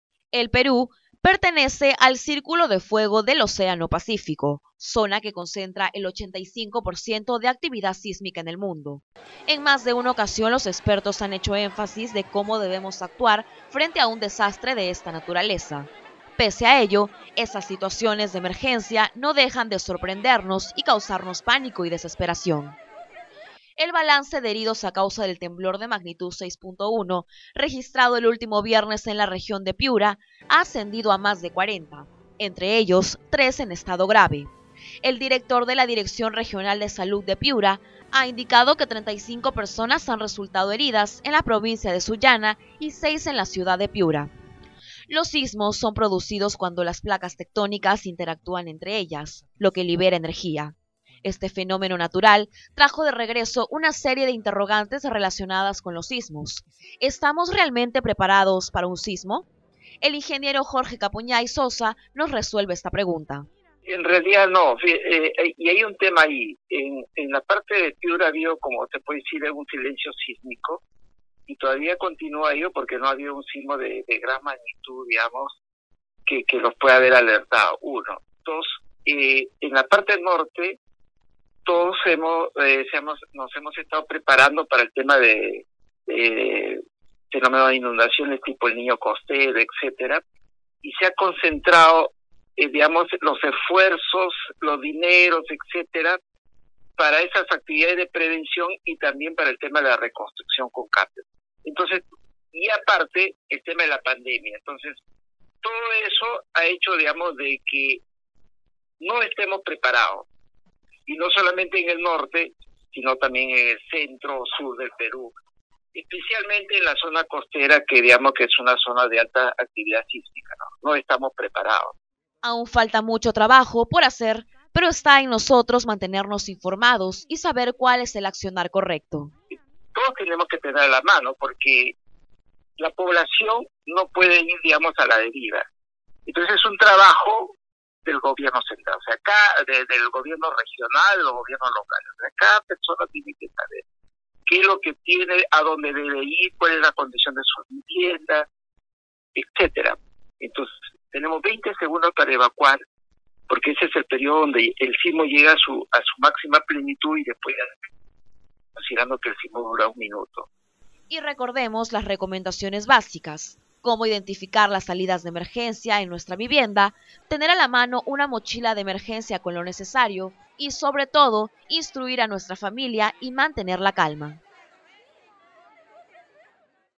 ¿Estamos realmente preparados para un sismo?
El sismo de 6.1 grado registrado el último viernes donde se reconocieron daños materiales y heridos en la ciudad de Sullana región Piura, nos trae la interrogante, ¿Estamos preparados realmente los peruanos para un movimiento telúrico? El ingeniero geológico Jorge Capuñay nos da la respuesta en la siguiente nota.

El Perú pertenece al “Círculo de Fuego del Océano Pacífico”, zona que concentra el 85% de actividad sísmica en el mundo. En más de una ocasión, los expertos han hecho énfasis en cómo debemos actuar frente a un desastre de esa naturaleza. Pese a ello, esas situaciones de emergencia, no dejan de sorprendernos y causarnos pánico y desesperación.

El balance de heridos a causa del temblor de magnitud 6,1 registrado este viernes en la región de Piura, ha ascendido a más de 40, entre ellos tres en estado grave. El director de la Dirección Regional de Salud de Piura, ha indicado que 35 personas han resultado heridas en la provincia de Sullana y seis en la ciudad de Piura.